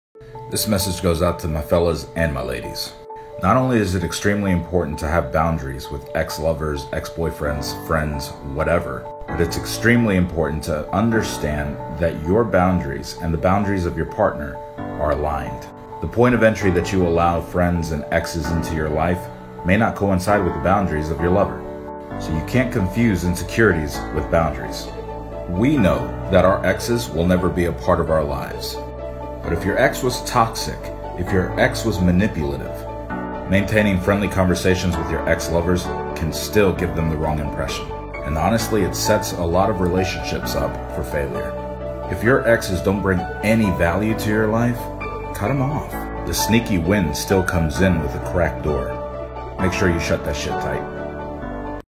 BOUNDARIES, EX'S, AND RELATIONSHIPS - CHRISTIAN ORTIZ
Christian Ortiz talks about the importance of boundaries.